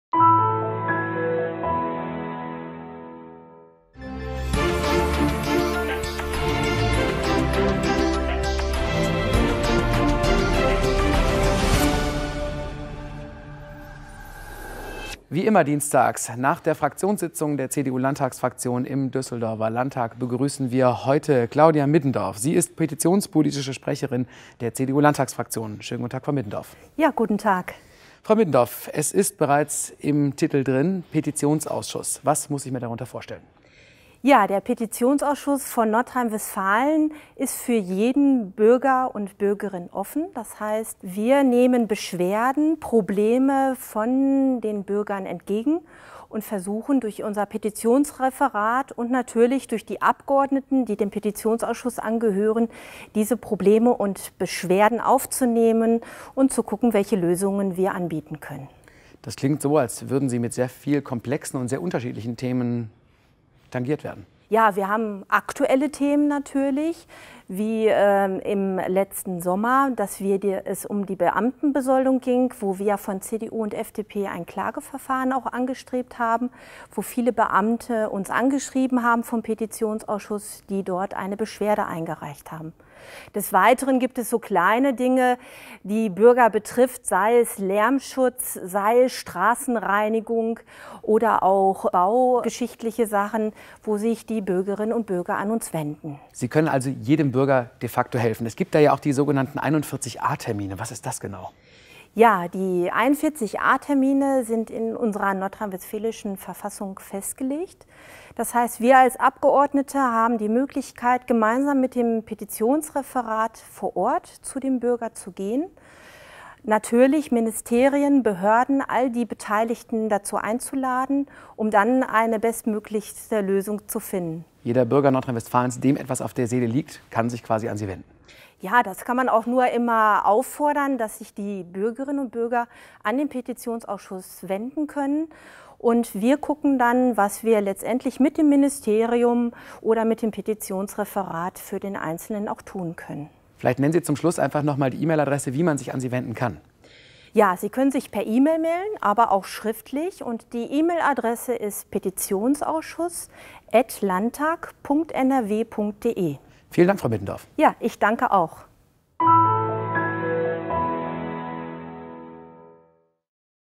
ansage: Claudia Middendorf zum Petitionsrecht für Bürgerinnen und Bürger in NRW
"Petitionen sind für jeden Bürger und jede Bürgerin möglich", sagt Claudia Middendorf, die petitionspolitische Sprecherin der CDU-Landtagsfraktion. Durch dieses Instrument ist es möglich, dass sich Bürger, die sich von einer Landesbehörde falsch behandelt fühlen, an den Petitionsausschuss wenden können. "In besonderen Fällen haben die Abgeordneten sogar die Möglichkeit direkt vor Ort zu den betroffenen Bürgern zu gehen, um eine gemeinsame Lösung zu finden."